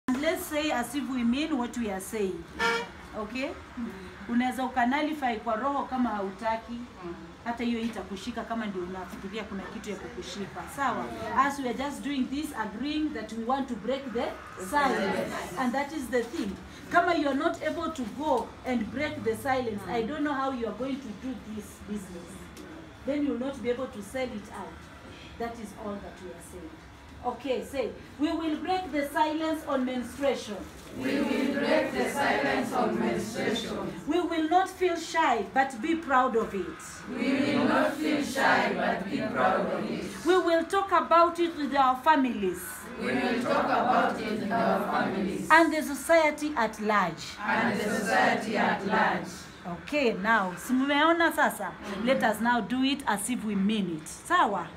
Break the Silence Pledge
While doing EVA Enterprise Development Training in Western Kenya, the group of grassroots leaders that convened joined us in taking the WSSCC "Pledge for Girls and Women":

I will break the silence on menstruation.
I will not feel shy: I will take pride.
I will spread the word outside and inside the home